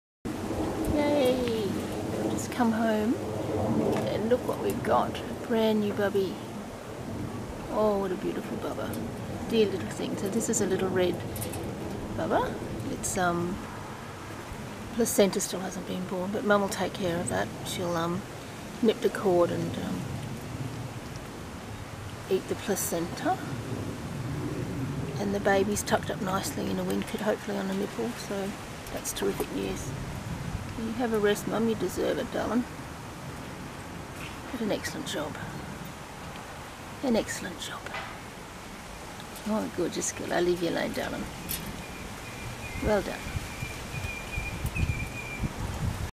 The perfection of a new born baby bat
Most Little Red flying-foxes give birth during the months of April/May and as Little Reds head north to winter and give birth, we sometimes have to over winter Reds who are not yet ready for release.
As a result we usually have some Little Red babies in care which will be released with their mothers once the Reds return to our area later in the year.